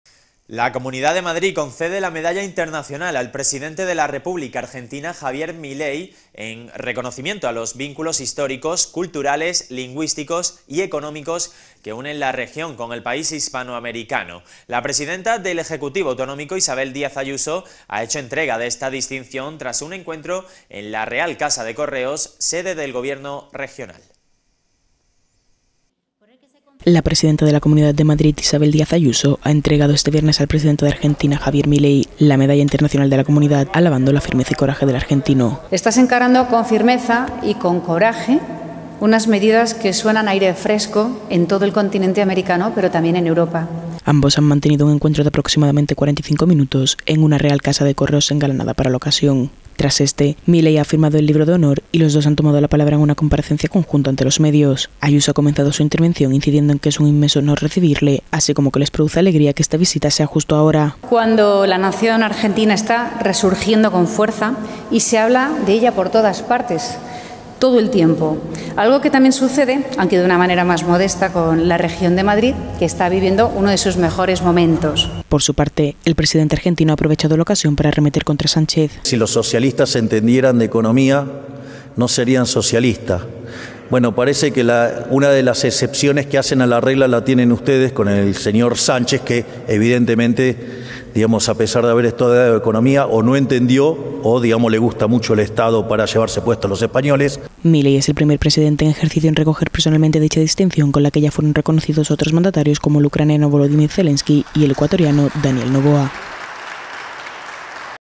🔴Noticia - La Comunidad de Madrid concede la Medalla Internacional a Javier Milei
La Comunidad de Madrid concede la Medalla Internacional al presidente de la República Argentina, Javier Milei, en reconocimiento a los vínculos históricos, culturales, lingüísticos y económicos que unen la región con el país hispanoamericano. La presidenta del Ejecutivo autonómico, Isabel Díaz Ayuso, ha hecho entrega de esta distinción tras un encuentro en la Real Casa de Correos, sede del Gobierno regional.
English:

The Community of Madrid awards the International Medal to the President of the Argentine Republic, Javier Milei, in recognition of the historical, cultural, linguistic and economic ties that unite the region with the Hispanic American country. The president of the regional Executive, Isabel Díaz Ayuso, presented this distinction after a meeting at the Real Casa de Correos, headquarters of the regional Government.

Live streaming